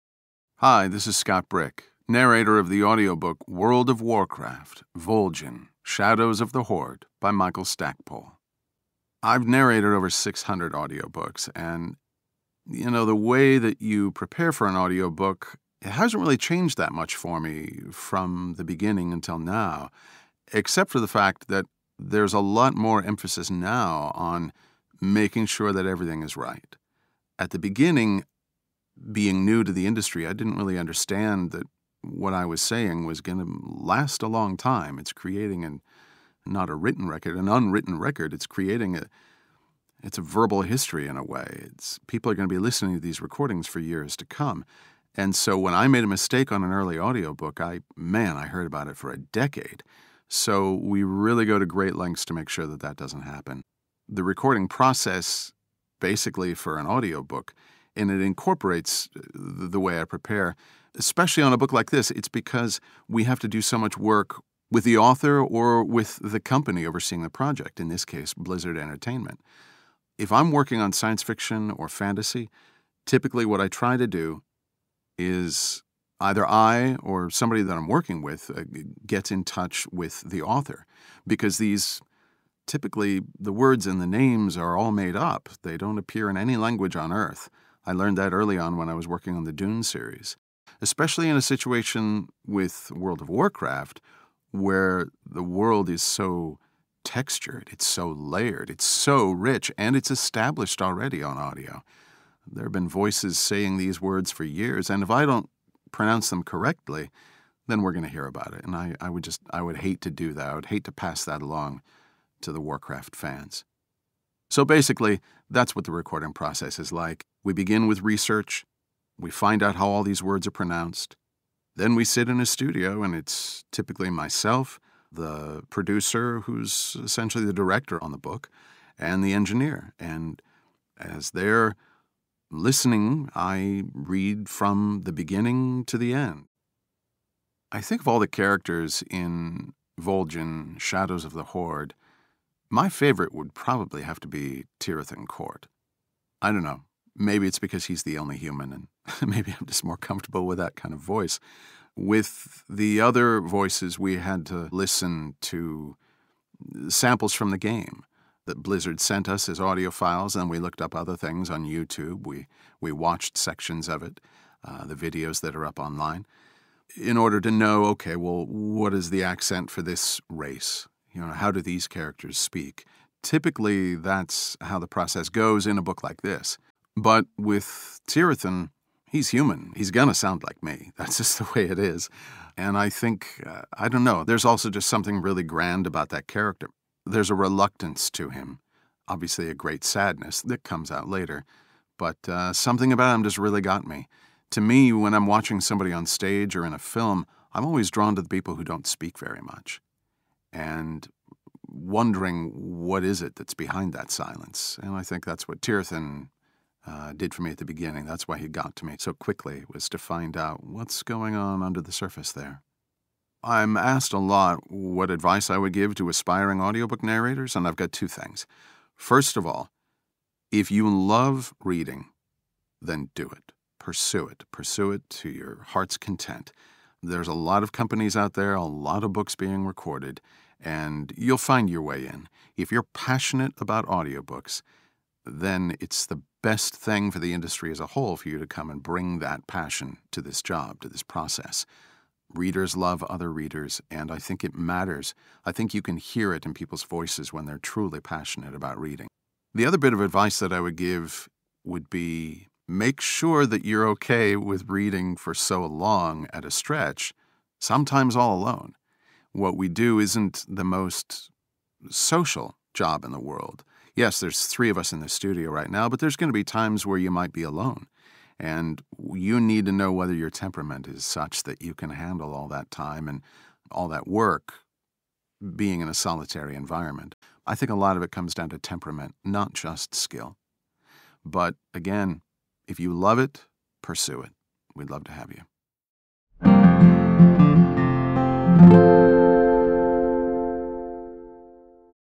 Scott Brick on narrating WORLD OF WARCRAFT: VOL'JIN: SHADOWS OF THE HORDE
Scott Brick discusses reading the World of Warcraft: Vol'jin: Shadows of the Horde audiobook by Michael A. Stackpole